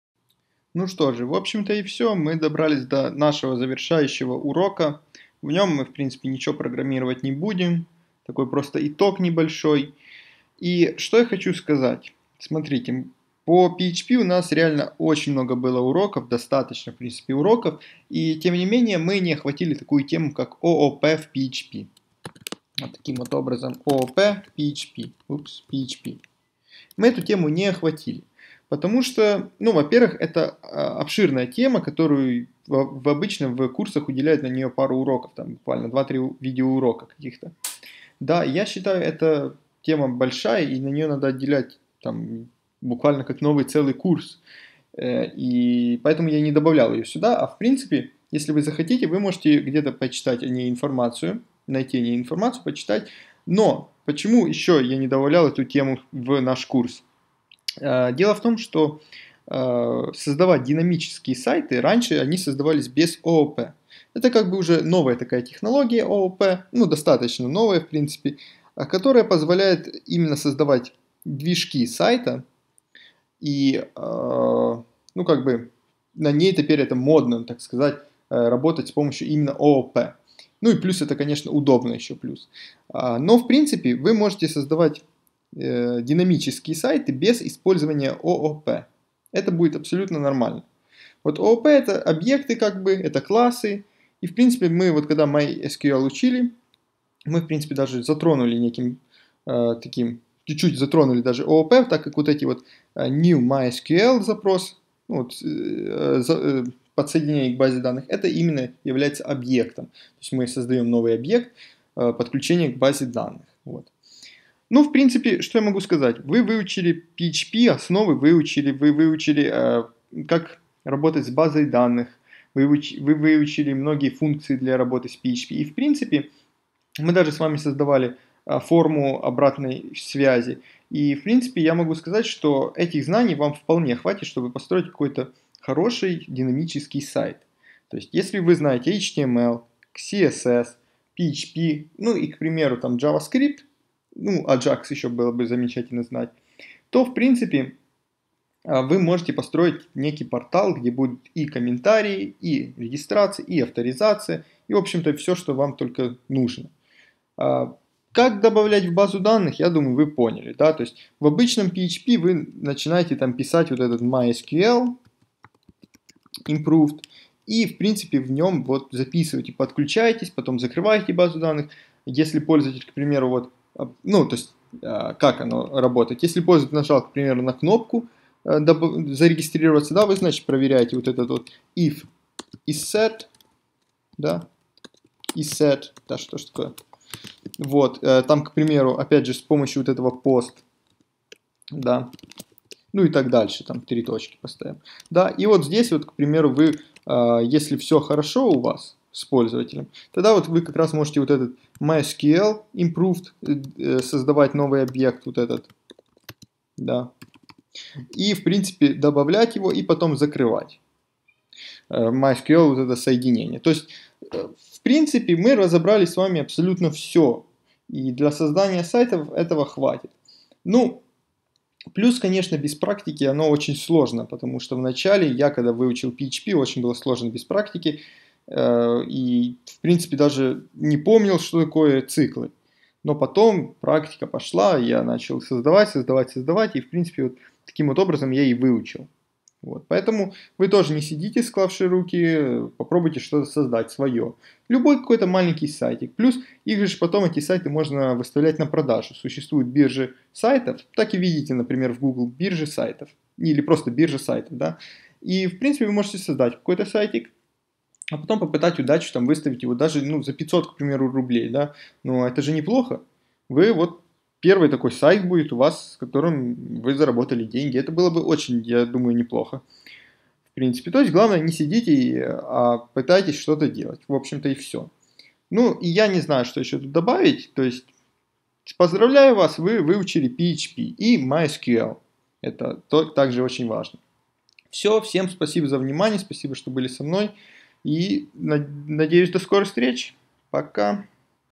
Изучение MySQL для начинающих | Урок #9 - Заключительный урок
Ура! Поздравляю вас, вы добрались до заключительного урока по изучению языка программирования PHP,  а также языка запросов к базе данных, языка MySQL.

* Кошельки WebMoney:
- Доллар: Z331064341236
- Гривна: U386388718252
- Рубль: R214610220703